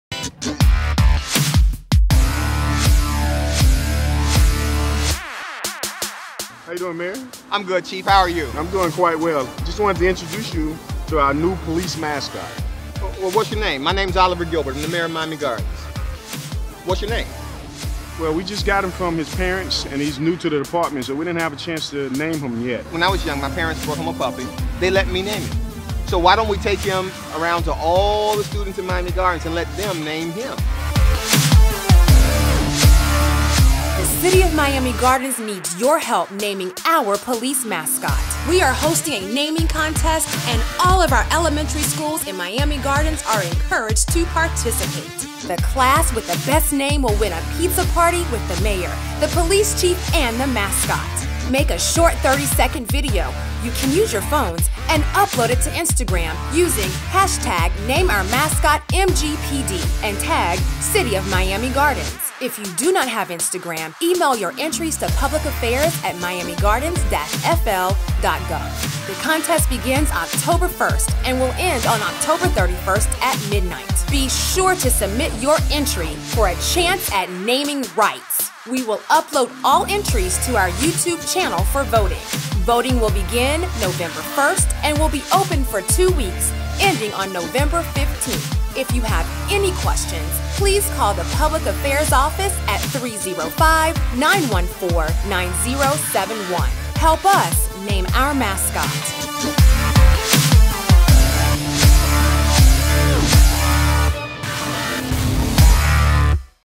Miami Gardens Mascot Naming Contest
The City of Miami Gardens needs your help naming our police mascot. We are hosting a naming contest and all of our elementary schools in Miami Gardens are encouraged to participate. The class with the best name will win a pizza party with the Mayor, the Police Chief and the Mascot.